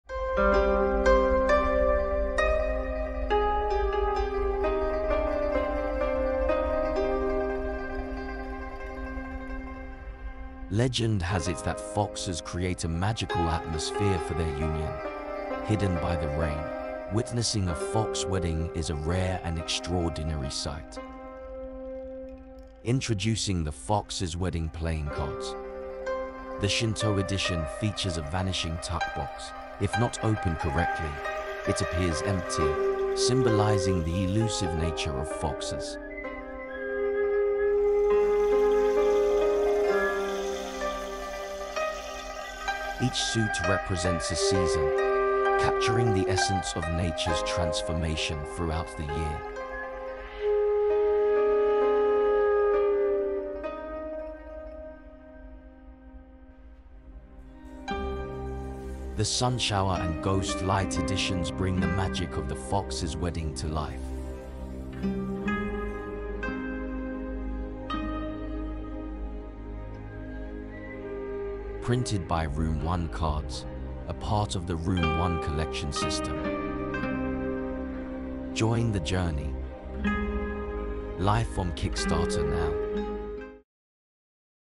Now on Kickstarter: The Fox's Wedding Playing Cards
The Fox's Wedding Playing Cards
Immerse yourself in a world of foxes, unravel the jigsaw puzzle, and treasure the beauty that lies within this deck of playing cards.